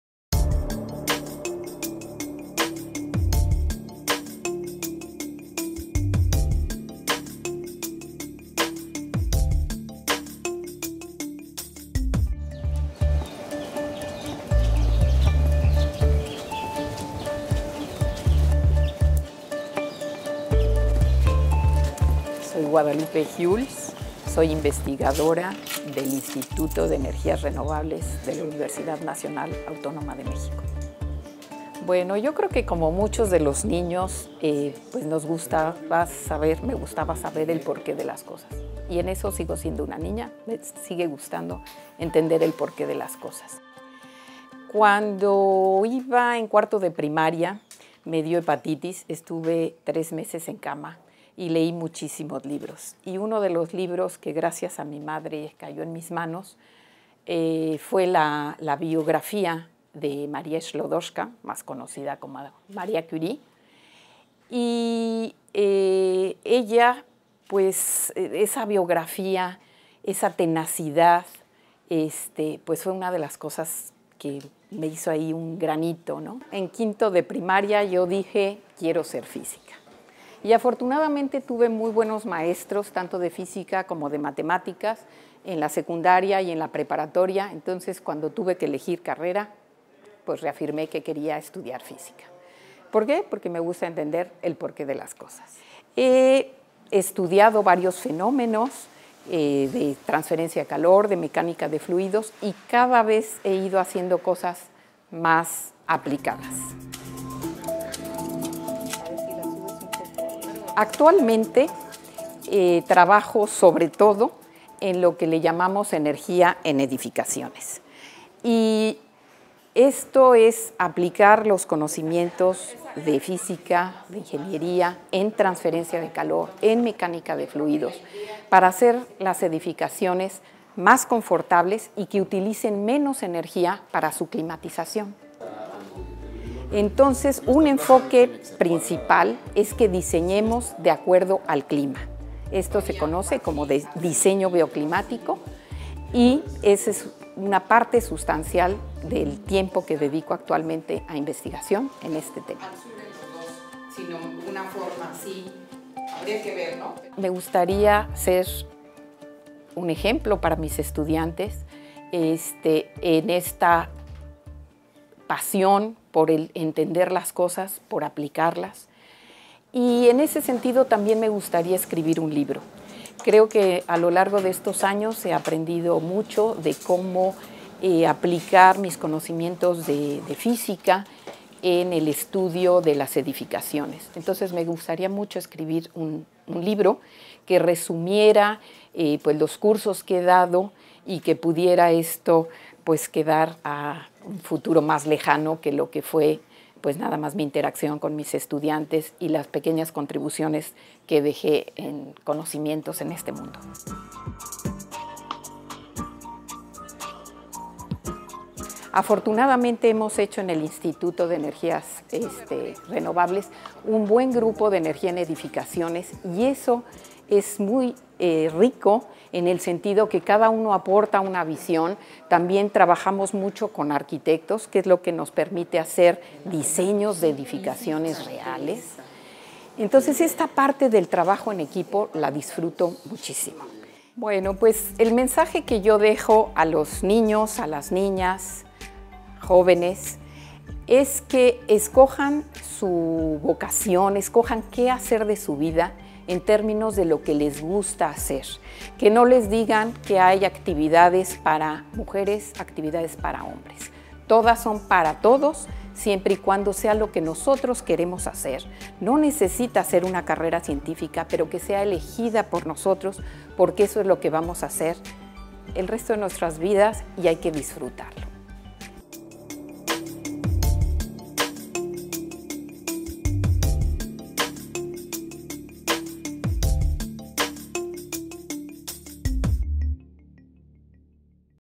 Dra Guadalupe Huelsz, Instituto de Energías Renovables UNAM - Científica/mente
Para despertar la vocación científica entre las y los jóvenes estudiantes, investigadores de diversas instituciones nos cuentan qué los motivó a dedicarse a la ciencia y por qué es importante su labor para nuestra sociedad.
En esta ocasión nos acompaña la Dra. Guadalupe Huelsz del Instituto de Energías Renovables de la UNAM Campus Morelos.